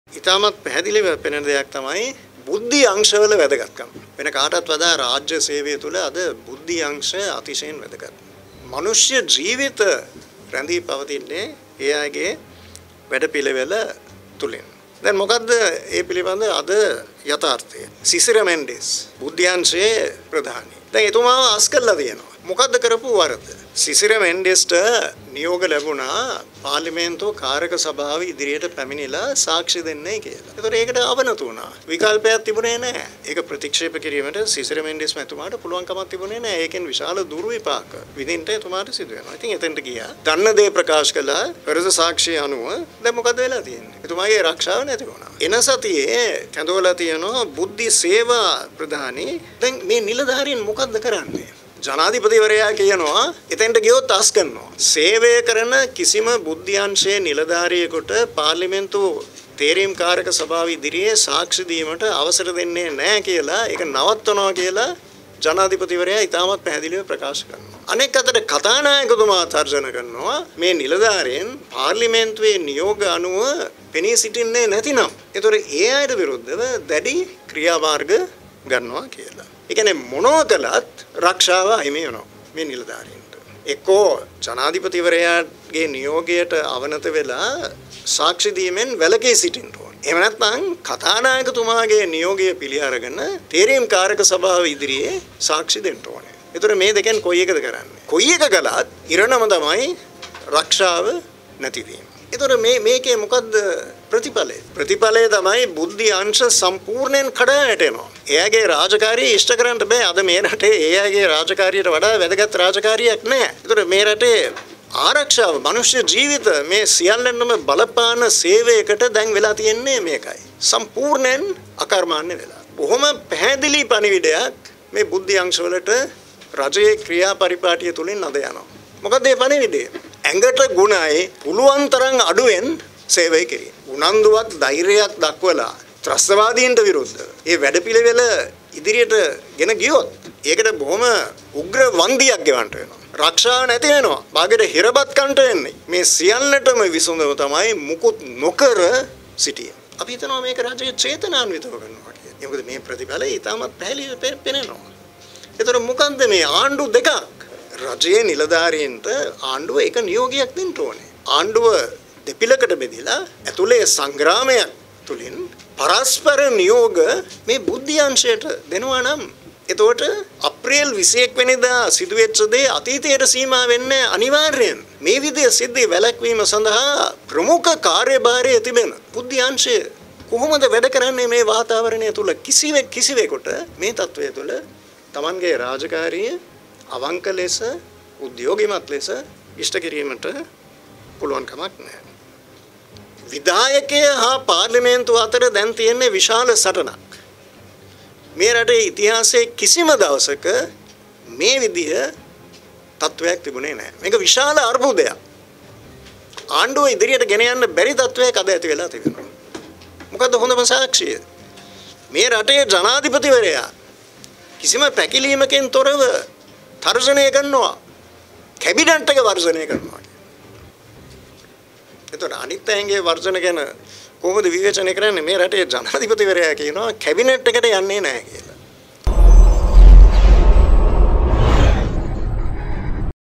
සිසිර මෙන්ඩිස් බුද්ධි අංශයේ ප්‍රදානියෙක් එතුමාව අස්කරලා තියෙනවා | Breaking.lk
සිසිර මෙන්ඩිස් බුද්ධි අංශයේ ප්‍රදානියෙක් එතුමාව අස්කරලා තියෙනවා
මොකක්ද එතුමා කරලා තියෙන වරද? -G.L. PEIRIS